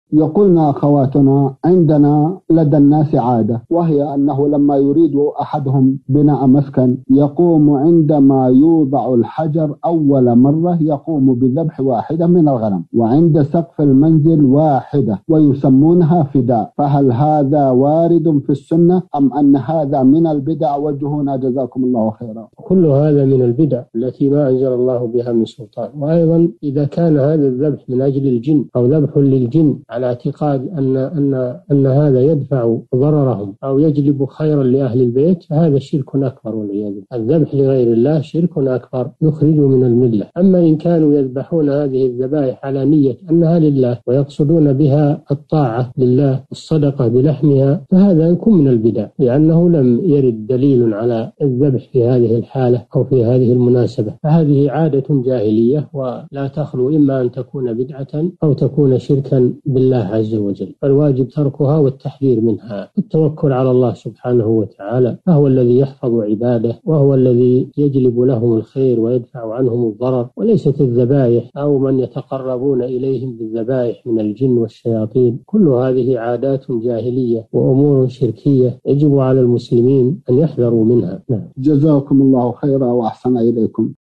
حكم الذبح عند بداية بناء البيت، وعند الانتهاء من بناء السقف، ويسمونها فدا؟ الشيخ صالح الفوزان
نص السؤال: عندنا لدى الناس عادة، وهي أنَّهُ لمَّا يريد أحدهم بناء مَسكن يقوم عندما يُوضع الحجر أول مرة، يقوم بذبح واحدة من الغنم، وعند سقف المنزل واحدة، ويسمونها فداء، فهل هذا وارد في السنة، أم أن هذا من البدع؟
السيرة الذاتية :
للاشتراك في قناة الشيخ صالح الفوزان

رئيس الفريق
لمشروع كبار العلماء